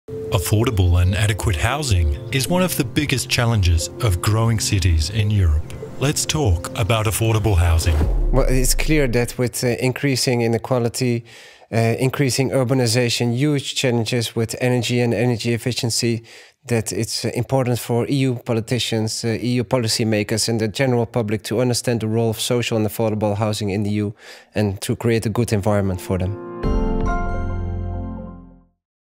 Let's Talk About Affordable Housing! Sébastien Garnier
Housing for All. Affordable Housing in Growing Cities in Europe
4-5 December 2018
Wiener Wohnen, Rosa-Fischer-Gasse 2
1030 Vienna

Which legal and financial conditions are necessary in Europe to increase investment in affordable housing?
Which benefits does affordable housing provide for European cities and the economy?

Mayors from different European cities, local politicians, housing experts, housing providers, tenants’ representatives and scientists will discuss these questions with the EU legislators comprehensively.
The findings of the EU Urban Agenda – Housing Partnership will be presented. The City of Vienna will offer study visits to present the Viennese model of social housing.